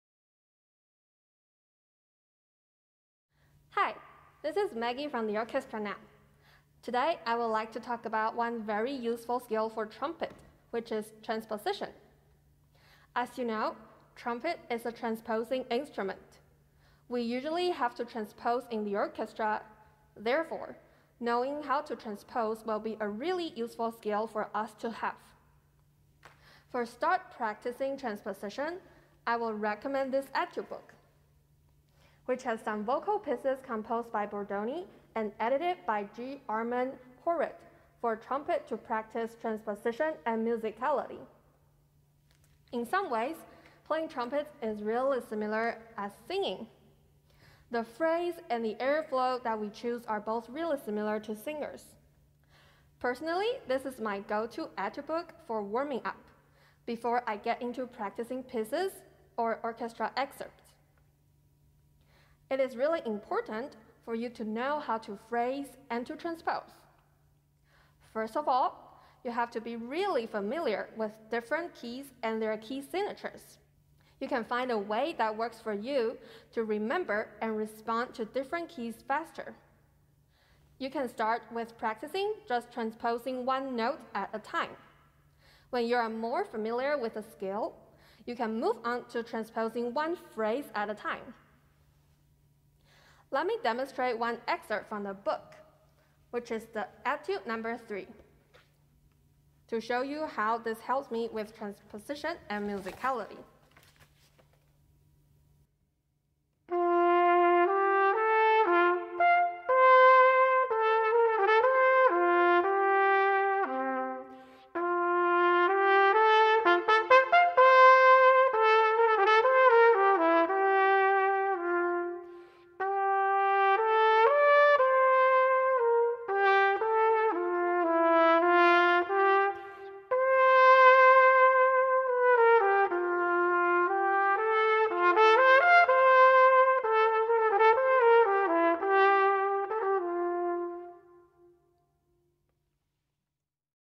Trumpet player Maggie Tsan talks about transposition.
The Orchestra Now Educational Library